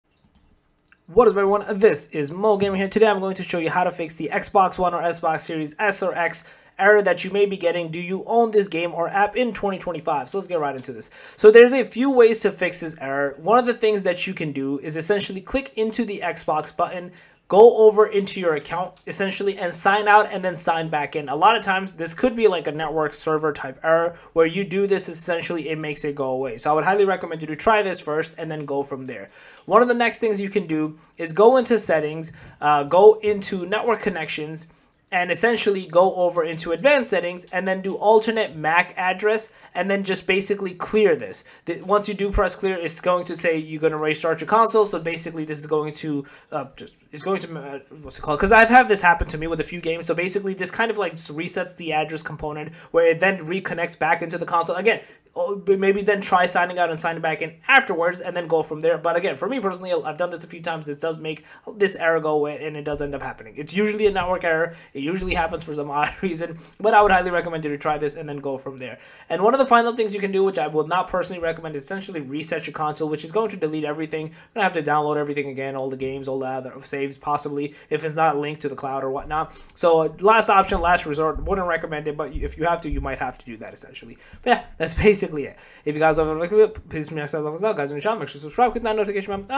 How to Fix Xbox One / Series X/S Error "Do You Own This Game or App" In 2025
In this video I talk about How to Fix Xbox One / Series X/S Error "Do You Own This Game or App" 2025